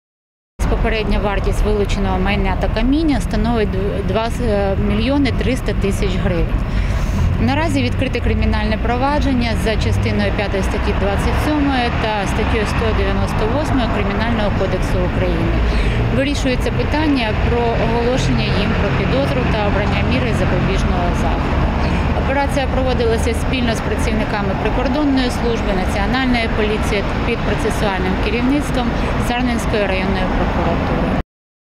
Співробітники СБУ викрили 3 липня групу скупників незаконно видобутого бурштину
Про це повідомляє прес-служба СБУ. Правоохоронці встановили, що четверо мешканців Дубровицького району скуповували у місцевих мешканців незаконно видобутий «сонячний» камінь для подальшого продажу закордон.
Під час санкціонованих обшуків за місцем проживання учасників нелегального бізнесу оперативники спецслужби вилучили майже п`ятдесят кілограмів бурштину-сирцю та вже оброблене каміння. Окрім того, в підсобному приміщенні одного з чловіків виявили знаряддя для видобутку та обробки каменю.